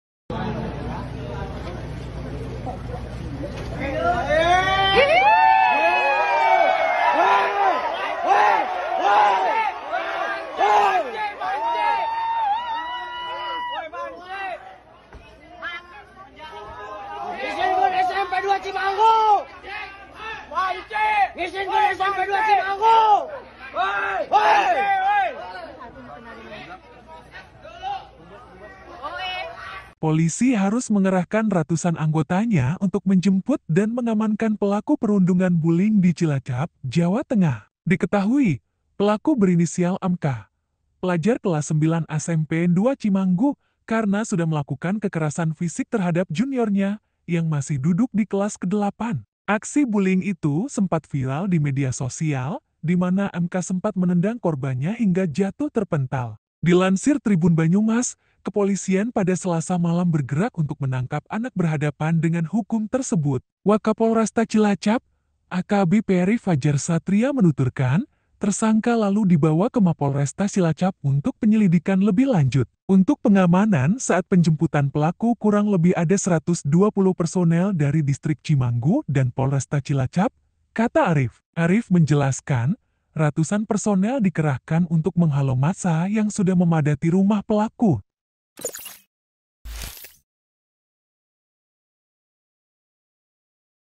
ALASAN RATUSAN POLISI Dikerahkan Jemput Pelaku Bullying Siswa SMP Cilacap: Hampir Diamuk Massa!
VP: Erric

SURYA.CO.ID - Polisi harus mengerahkan ratusan anggotanya untuk menjemput dan mengamankan pelaku perundungan (bullying) di Cilacap, Jawa Tengah. Diketahui, pelaku berinisial MK, pelajar kelas 9 SMPN 2 Cimanggu, karena sudah melakukan kekerasan fisik terhadap juniornya, yang masih duduk di kelas 8. Dilansir TribunBanyumas, kepolisian pada Selasa malam (26/9/2023) bergerak untuk menangkap anak berhadapan dengan hukum tersebut. Wakapolresta Cilacap, AKBP Arif Fajar Satria menuturkan, tersangka lalu dibawa ke Mapolresta Cilacap untuk penyelidikan lebih lanjut.

"Untuk pengamanan (saat penjemputan pelaku) kurang lebih ada 120 personel dari distrik Cimanggu dan Polresta Cilacap," kata Arif. Arif menjelaskan, ratusan personel dikerahkan untuk menghalau massa yang sudah memadati rumah pelaku.